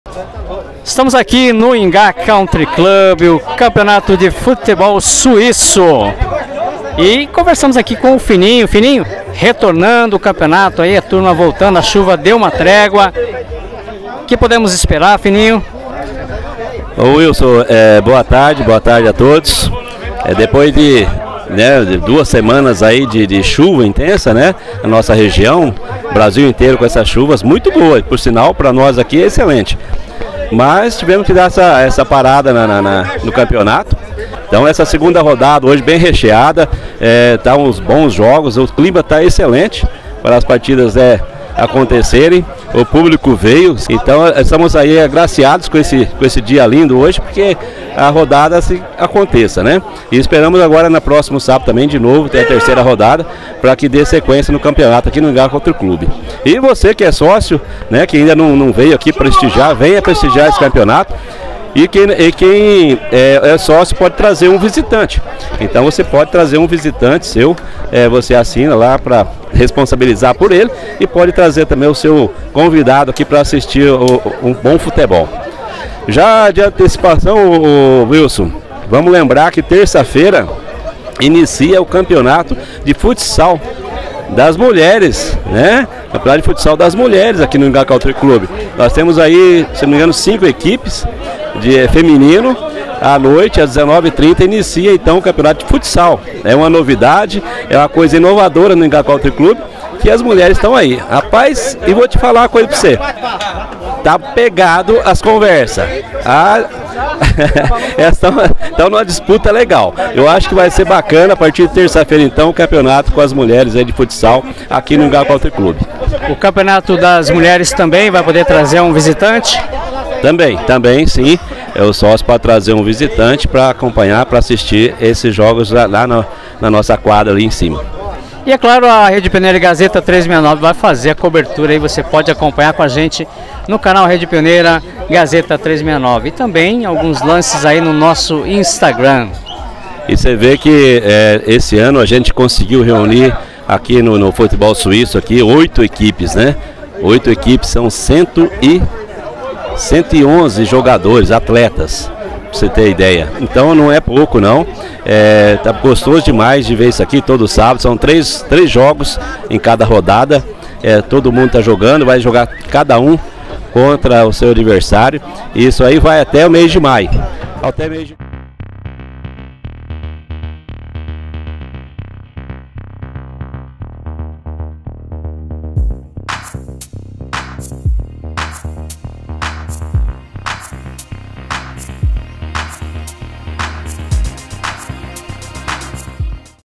Campeonato de Futebol Suíço e Futsal Feminino em destaque no Ingá Country Club de Andirá
Fininho fala da 2ª Rodada do Campeonato de Futebol Suíço e Futsal Feminino no Ingá Country Club de Andirá